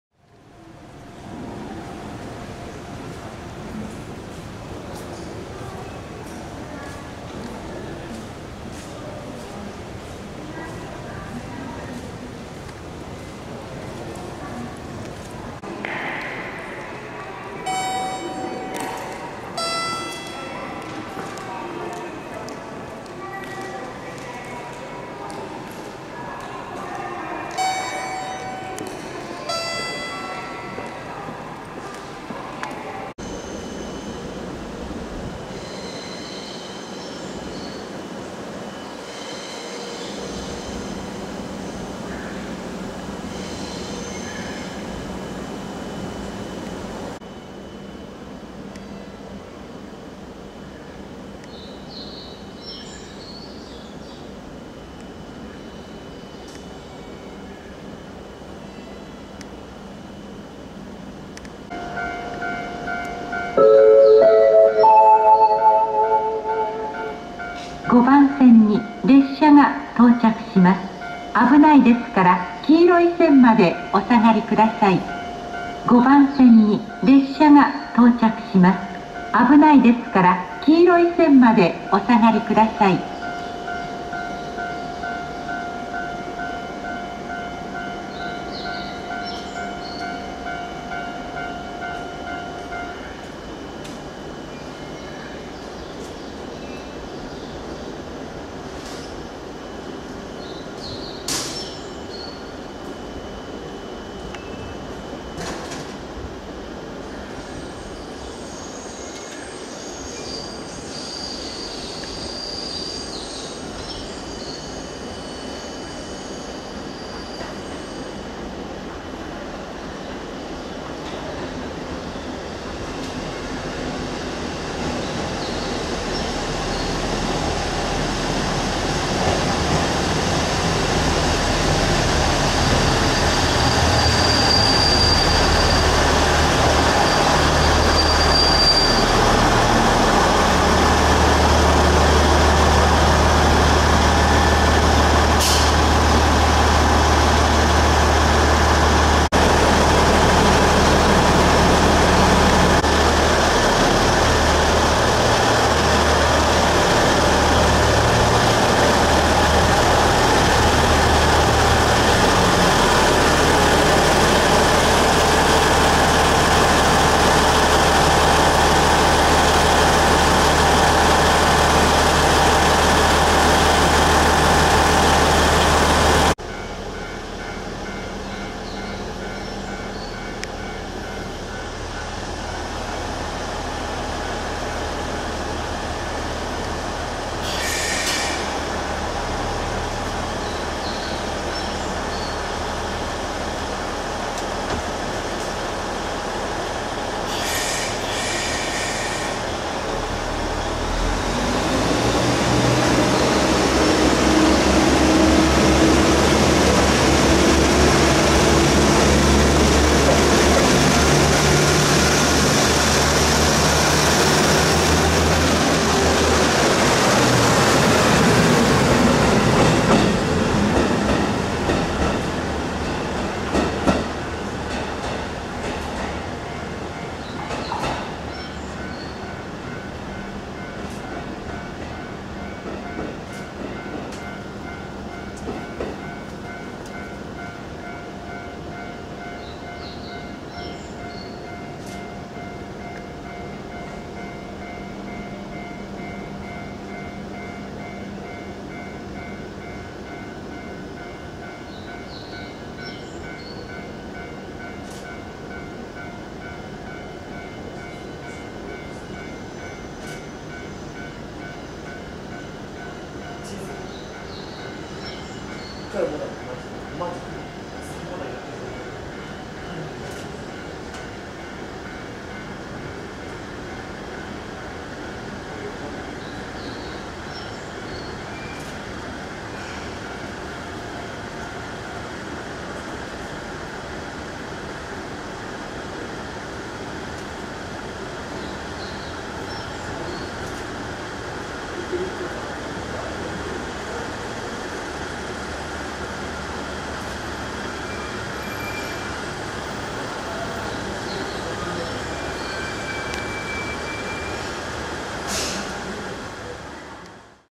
終電時間帯の長野駅に、仕事を終え空荷で到着した、キヤE195系レール輸送車。
令和4年6月9日(木)夜の長野駅へ、キヤE195系レール輸送車2両が、空荷の状態で到着しました。
他の場所で、レール輸送が行われたと思われます。
長野駅にて数分停車後に、長野総合車両センターへ向かいました。